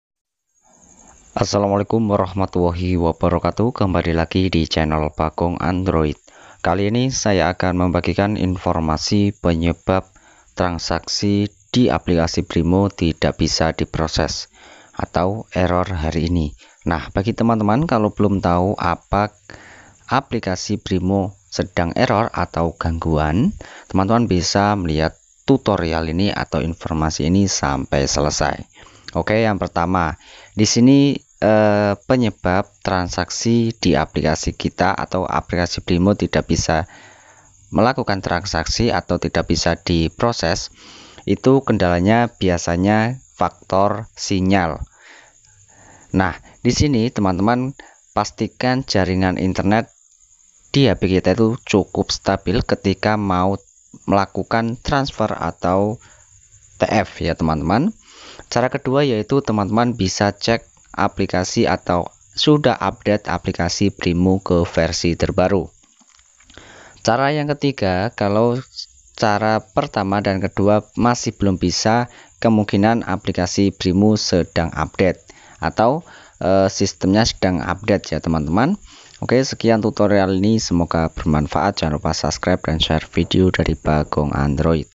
Penyebab Transaksi Tidak Bisa Diproses BRImo Gangguan Hari Ini
Halo teman-teman kembali lagi di channel Bagong Android kali ini saya akan membagikan informasi Penyebab Transaksi Tidak Bisa Diproses BRImo Gangguan Hari Ini

Nah Penyebab Transaksi Tidak Bisa Diproses BRImo Error tonton terus video ini sampai selesai Agar kalian paham